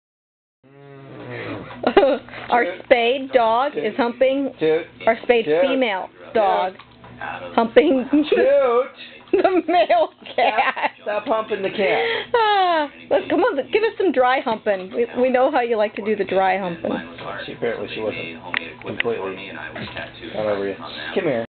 Tootie humping the cat
Tootie, a FEMALE spayed basset hound, humping Kiddo, a neutered male cat (and her brother).